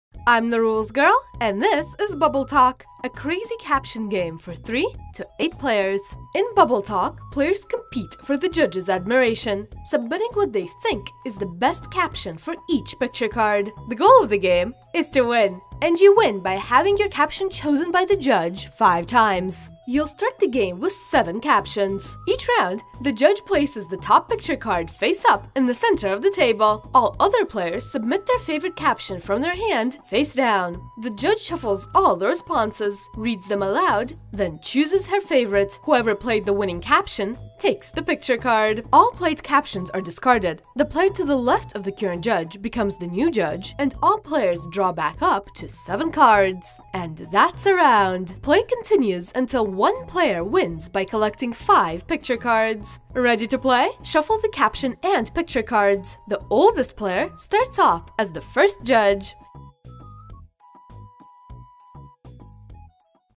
How to Play Bubble Talk in 2 Minutes - The Rules Girl
The Crazy Caption Board Game!​

It's the hilarious picture-caption matching game that's perfect for the entire family! The judge draws a picture card, then players select the best caption card from their hands and place it face down. The player with the most gut-busting card wins the round. For 3 to 8 players, ages 8 and up.